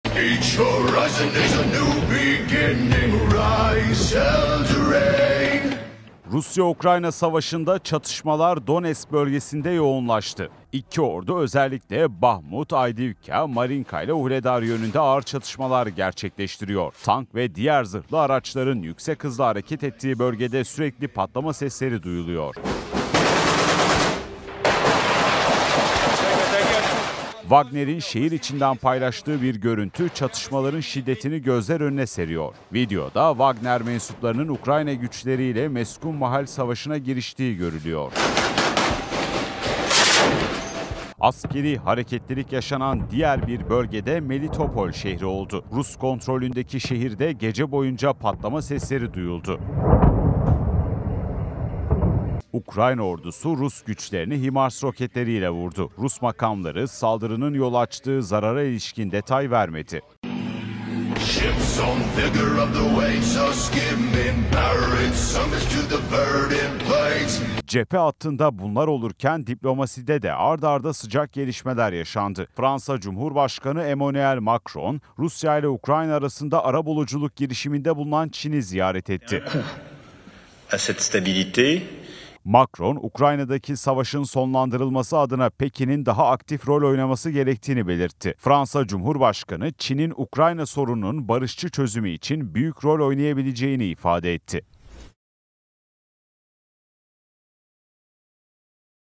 Rusya Ukrayna Savaşında ABD'nin HIMARS Roketleri Sahada! Çatışmalar Donetsk'te Kilitlendi-TGRT Haber
Rusya Ukrayna savaşı Donetsk bölgesinde kilitlenmiş durumda. Rus paralı asker grubu Wagner üyelerinin Bahmut şehrinde girdiği bir çatışma, savaşın şiddetini bir kez daha gözler önüne serdi. Ukrayna ise Rusları HIMARS roketleriyle vurdu.